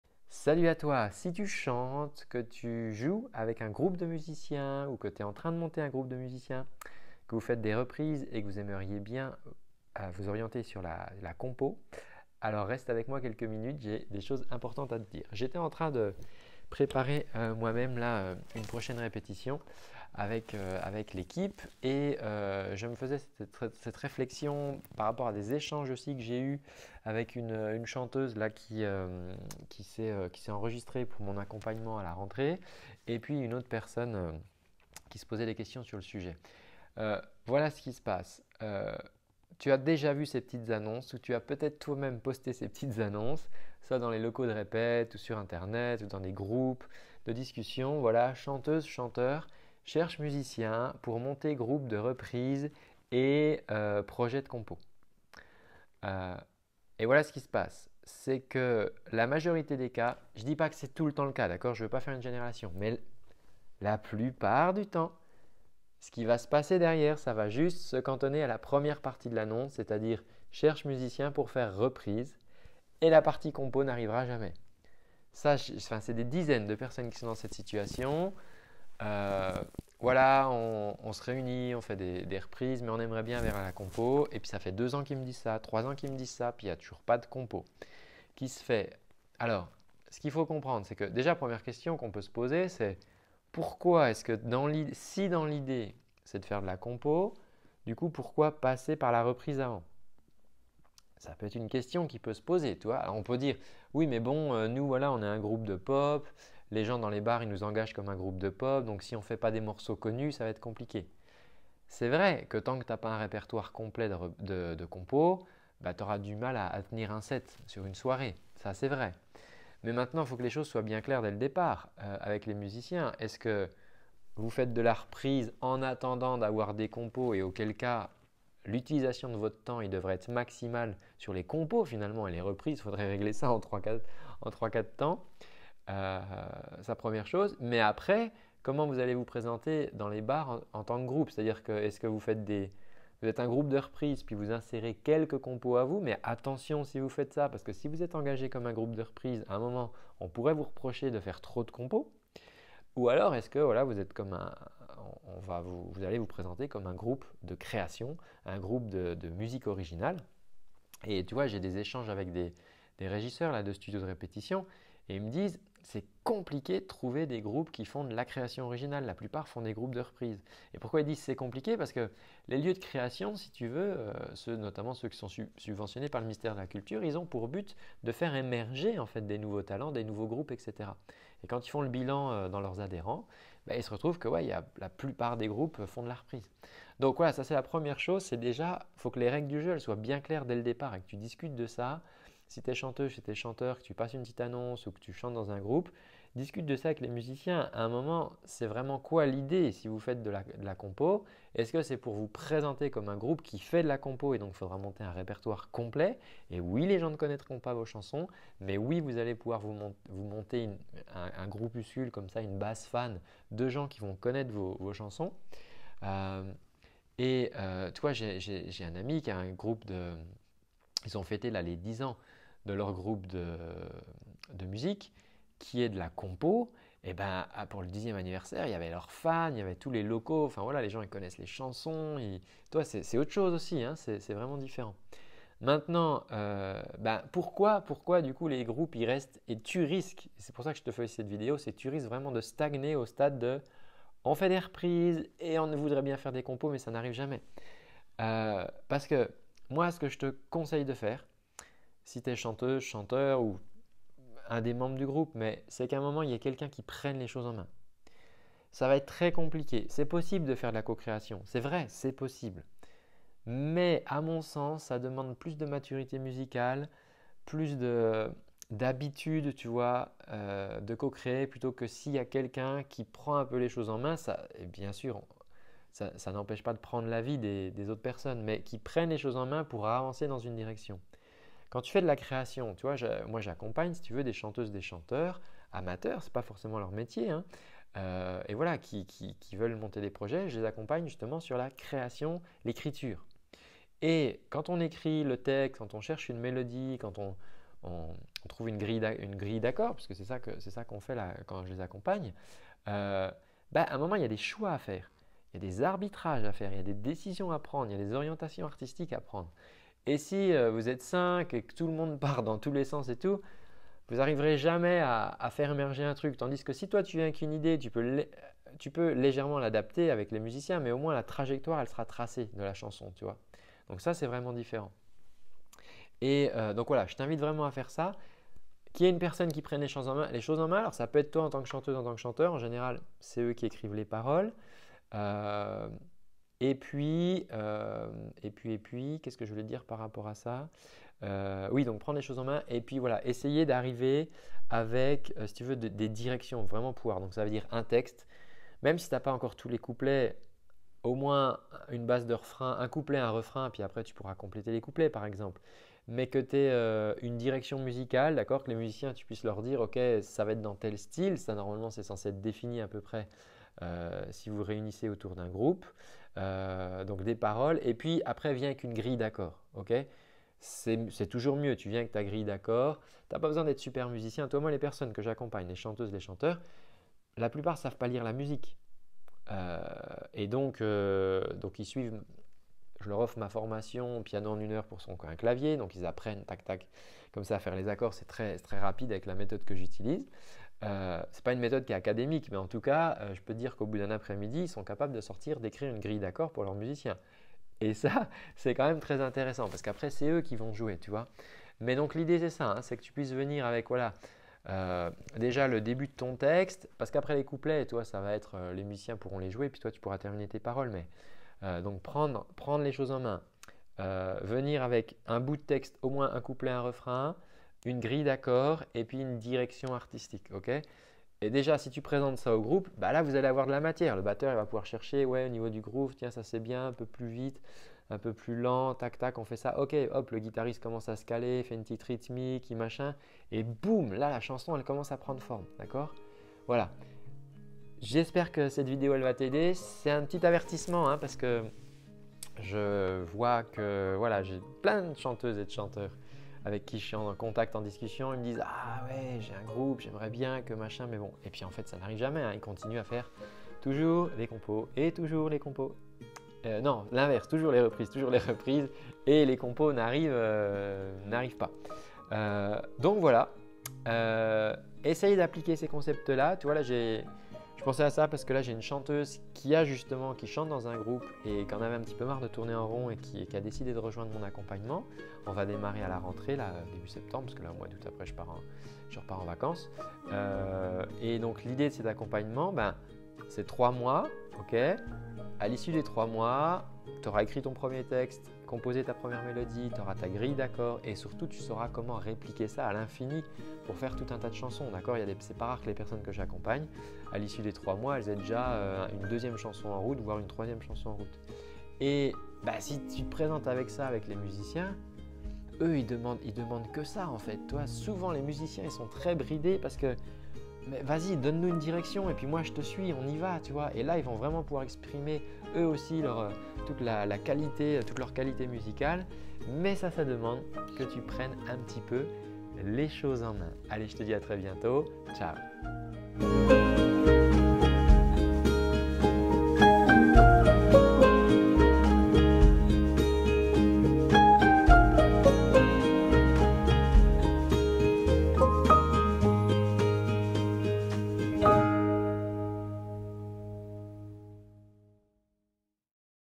Composer en groupe ?
Monter un groupe pour faire des reprises pour commencer avec le projet d’aller ensuite sur des compos ?

Tu n’es pas le seul chanteur à faire ça.

Voilà le problème que tu vas rencontrer ( tu es peut être déjà dedans )

Et les premières actions à mettre en place pour t’éviter ce qui arrive à 90% de ce genre projets

Le résultat ?